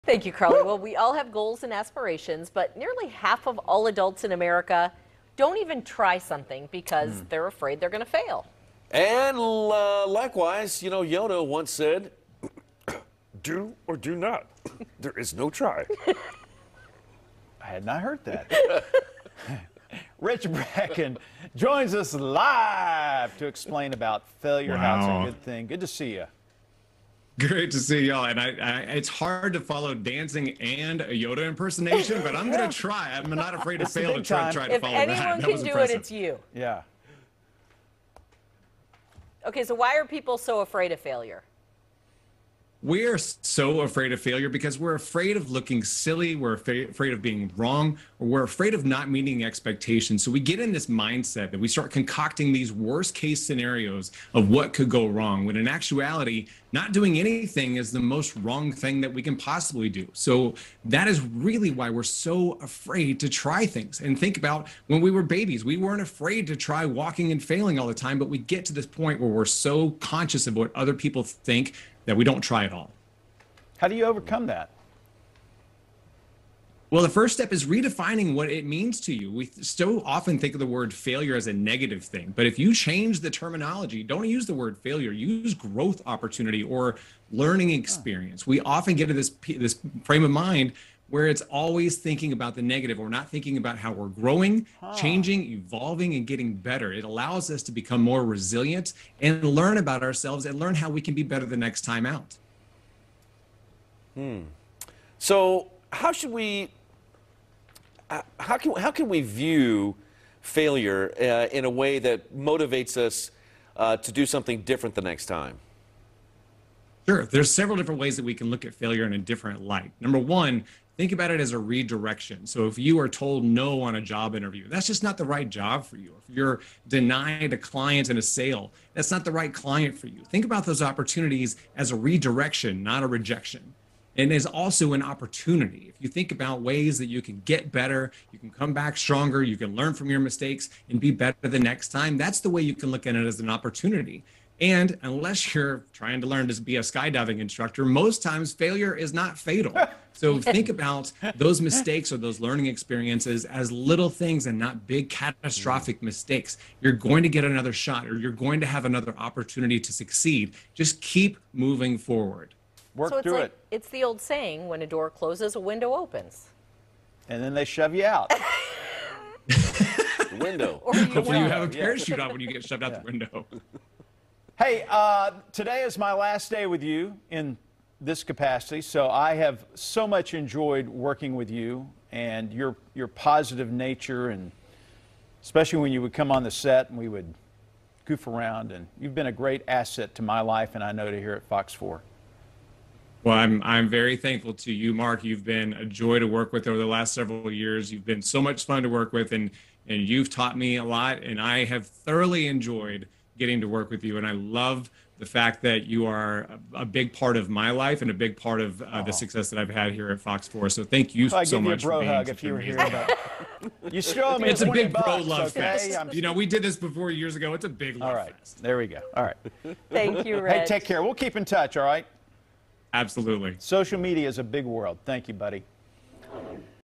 Overcoming the Fear of Failure
We all have goals and aspirations of things we’d like to achieve or accomplish, but nearly half of adults in America don’t even start their goal pursuit due to fear of failure. So, imagine what could happen we viewed failure as a good thing.

In this segment on Fox 4 Kansas City, I share some insights on how you can completely reframe your view of failure to make it a positive element when it happens instead of a complete shutdown.